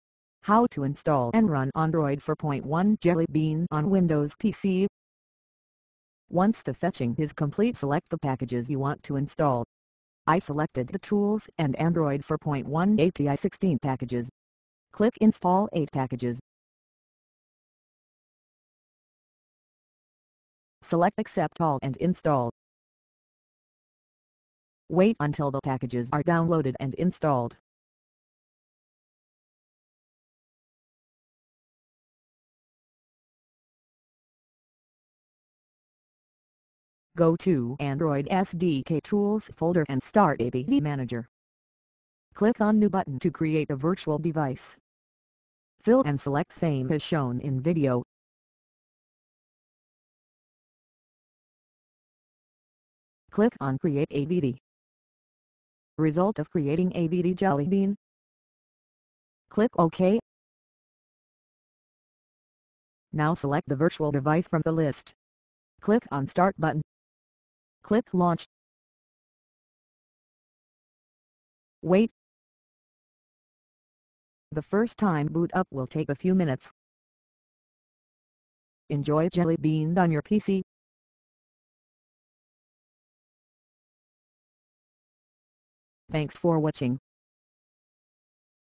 Run Android 4.1 Jelly Bean on Windows PC [Video Tutorial]
Here's a step by step guide on how you'll be able to install the Android SDK and run Jelly Bean on your Windows PC.